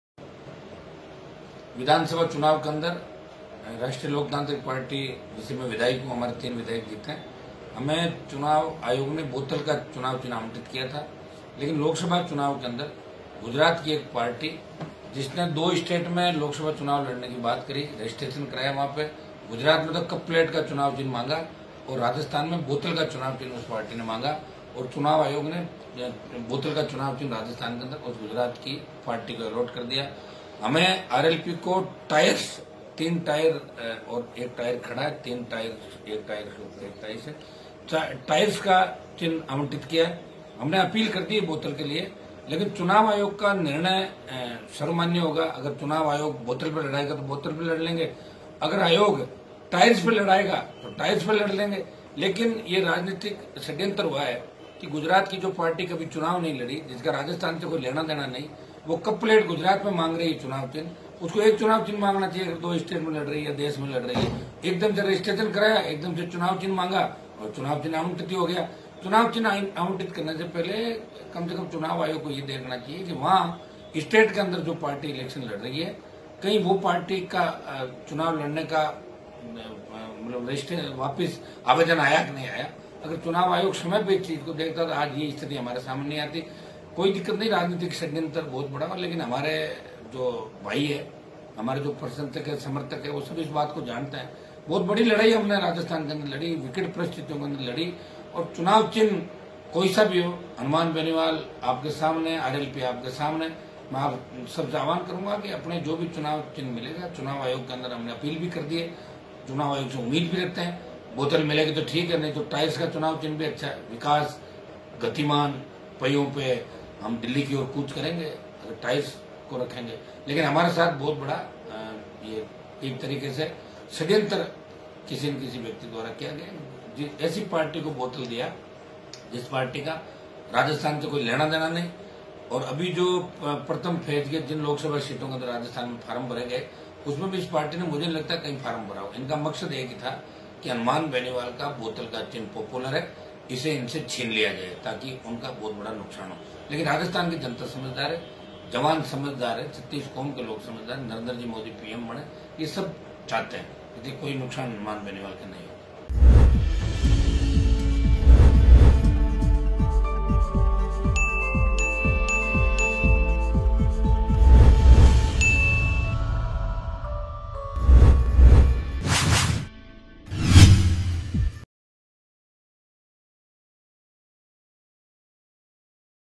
AJMER NEWS | बेनीवाल ने कहा हमारा लक्ष्य नरेंद्र मोदी को प्रधानमंत्री बनाना है | MTTV INDIA
बेनीवाल ने कहा हमारा लक्ष्य नरेंद्र मोदी को प्रधानमंत्री बनाना है,
 कहा कांग्रेस द्वारा फैलाई जा रही भ्रांतियों में नही उलझे
हनुमान बेनिवाल बीजेपी प्रत्याशी, नागौर लोकसभा